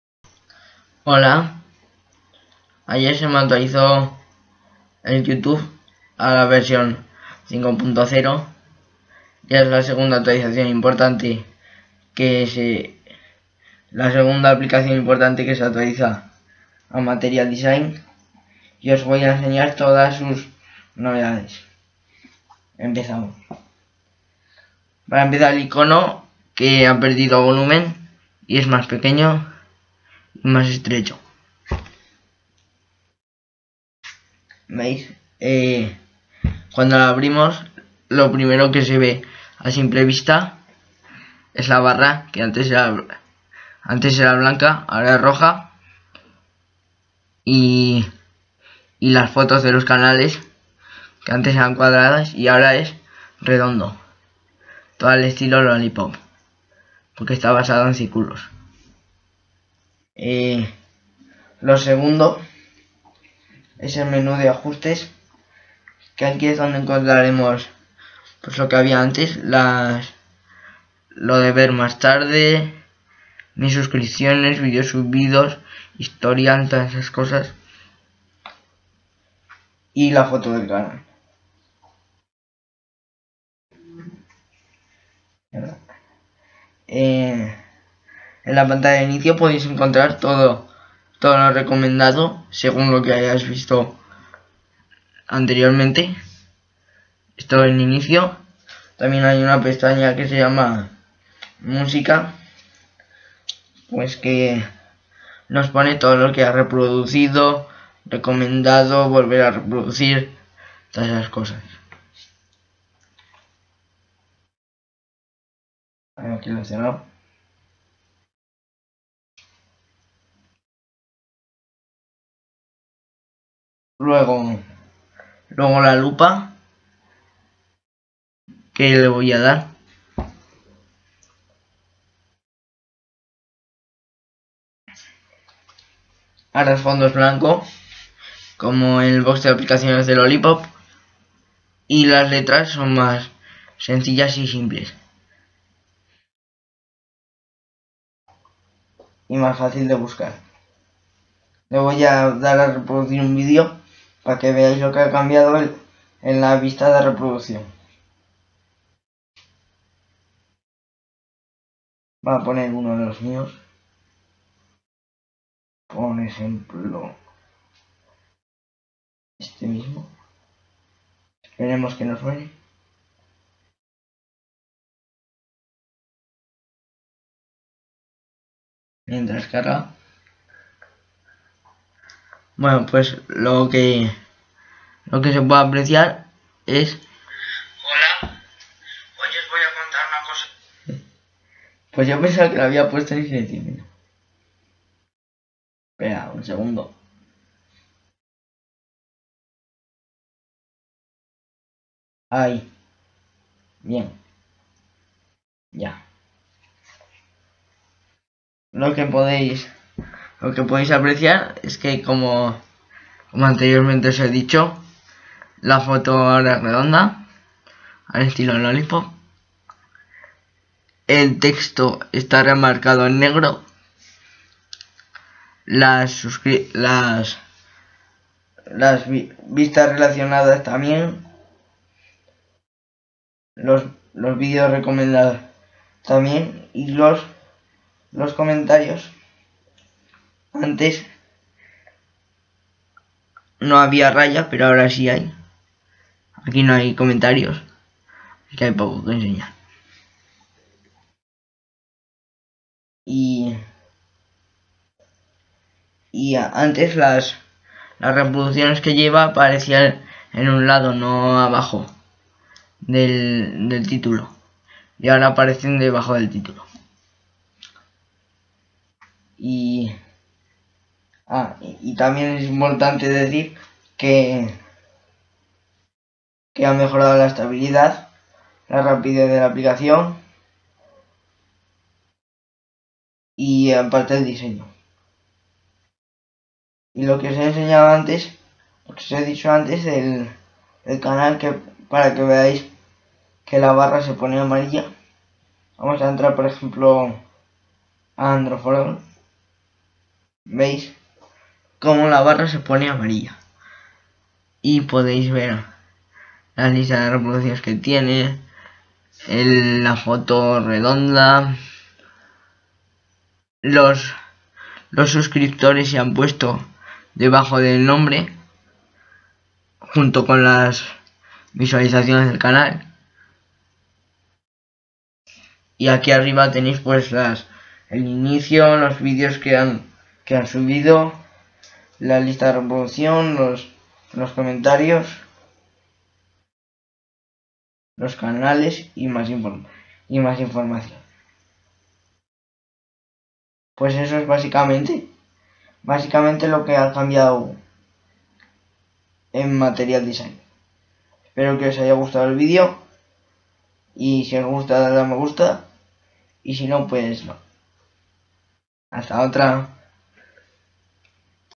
YouTube se actualiza al diseño 5.0
La primera app en actualizarse fue Telegram, ahora le toca el turno a YouTube. Espero que os guste este nuevo diseño.